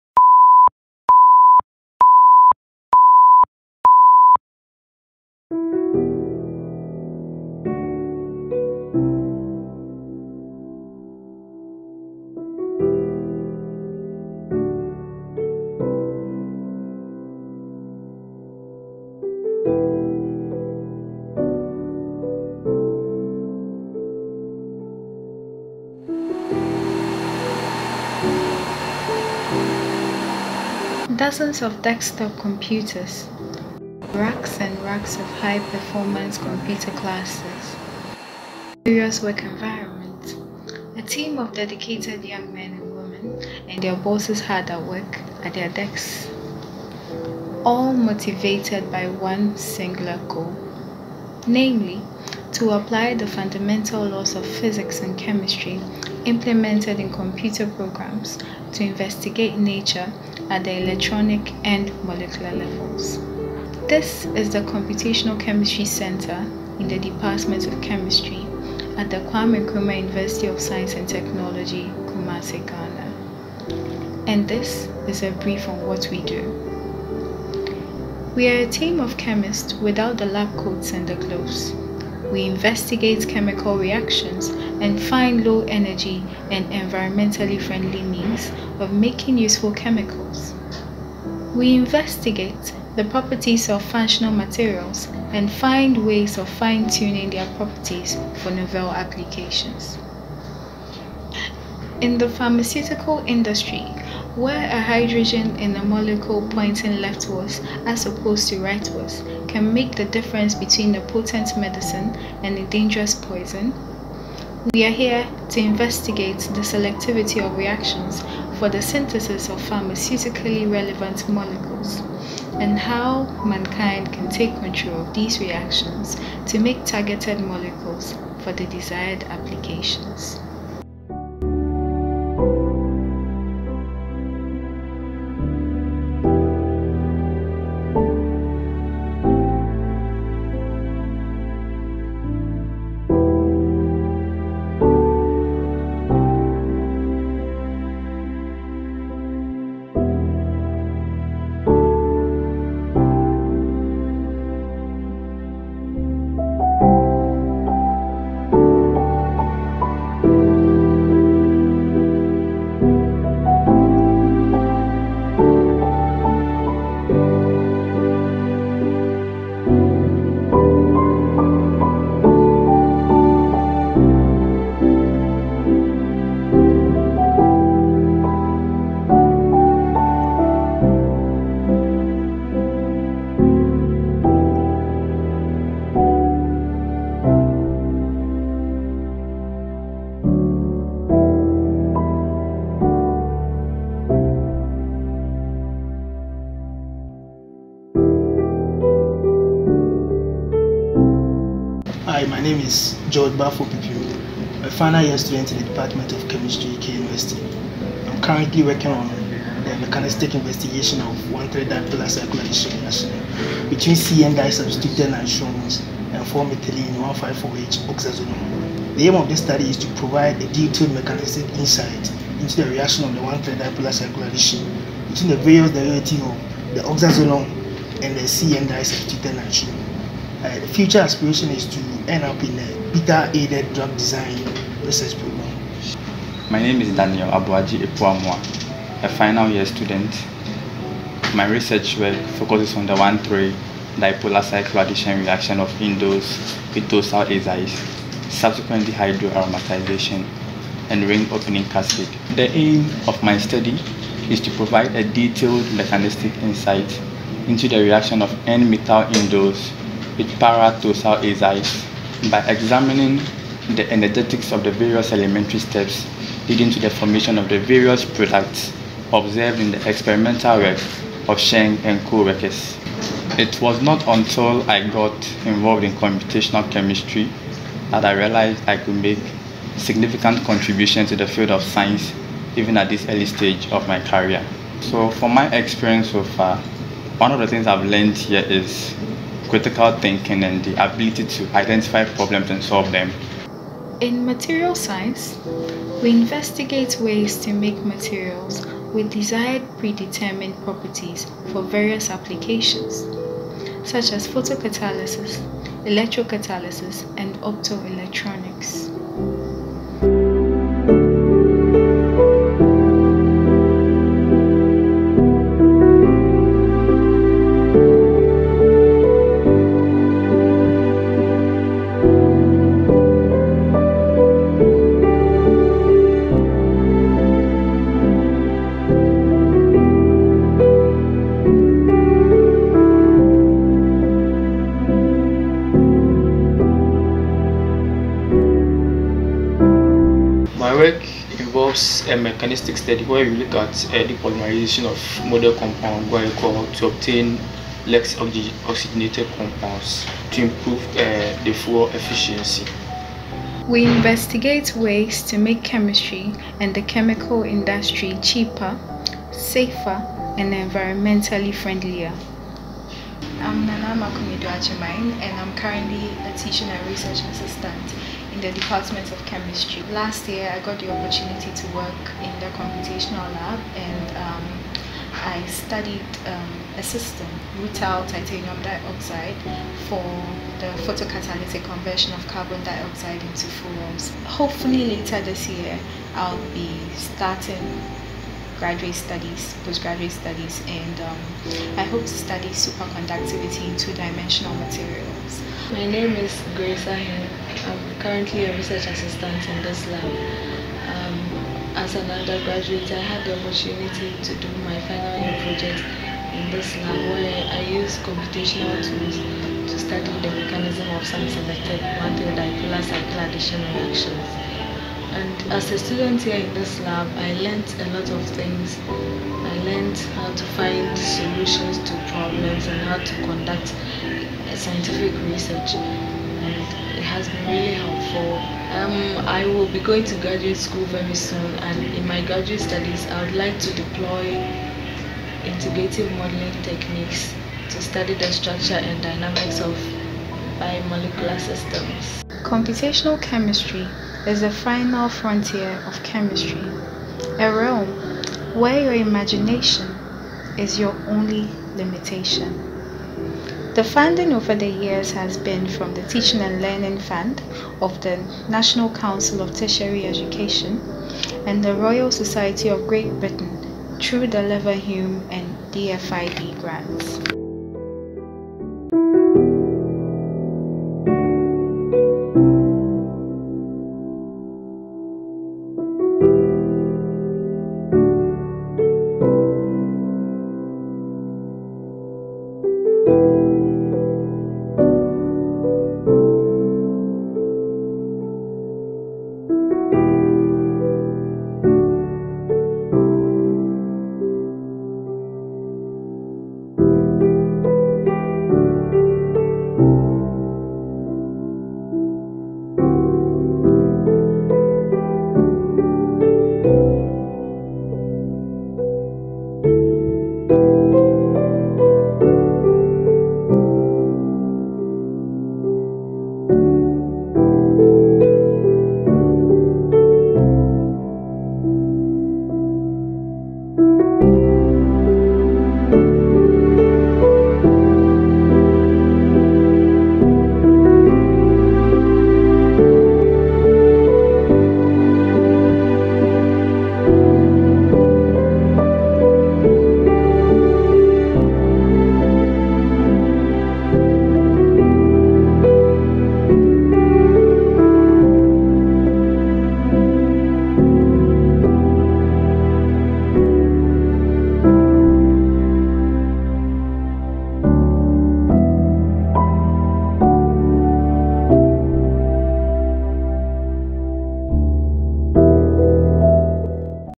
KNUST Computational Chemistry 2020
This short video tells the story of computational chemistry research in the Department of Chemistry at the Kwame Nkrumah University of Science and Technology, Kumasi, Ghana.